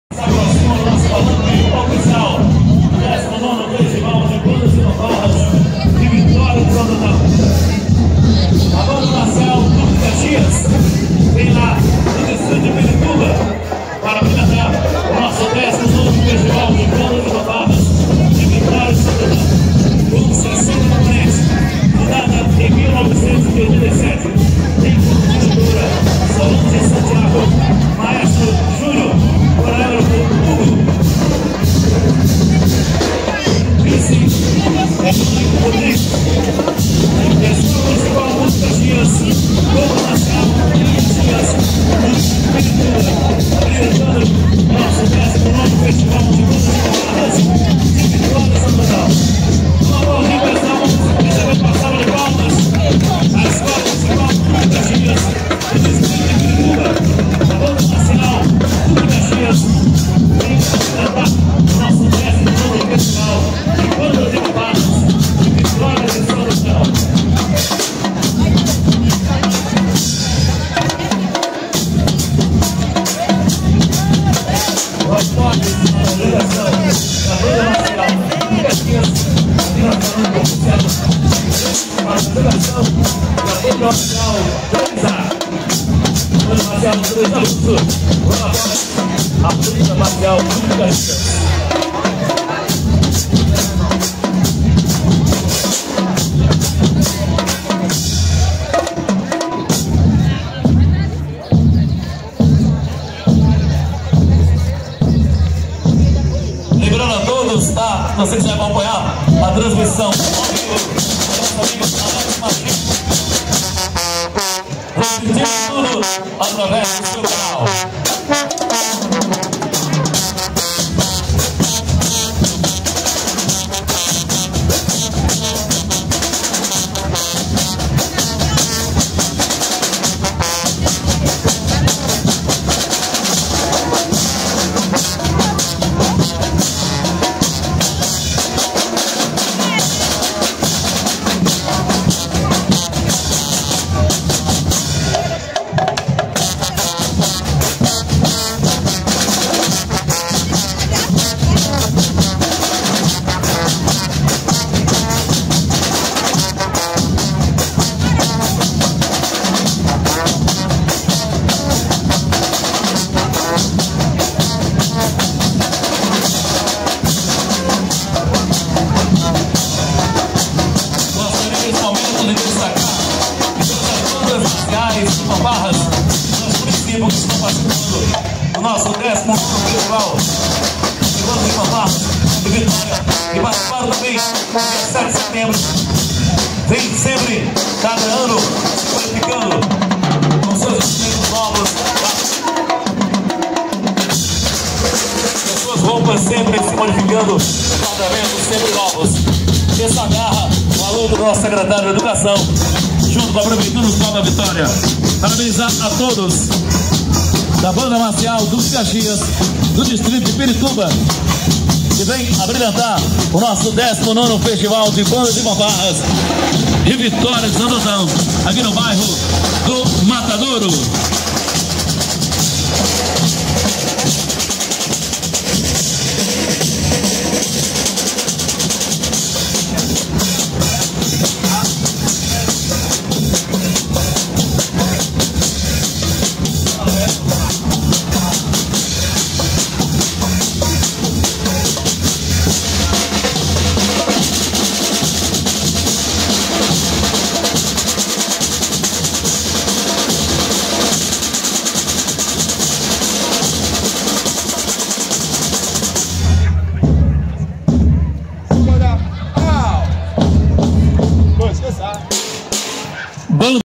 BANDA MARCIAL DUQUE DE CAXIAS 2024 NO FESTIVAL DE BANDAS E FANFARRAS DA VITÓRIA DE SANTO ANTÃO 2024
COBERTURA DO EVENTO👇👇
Parceria com Equipe de MÍdia Alex Marching Band

* REALIZAÇÃO : DIMAS CÉLIO

Evento Realizado no dia 15 de Setembro de 2024
Município da Vitória de Santo Antão - PE.
19° FESTIVAL DE BANDAS E FANFARRAS DA VITÓRIA 2024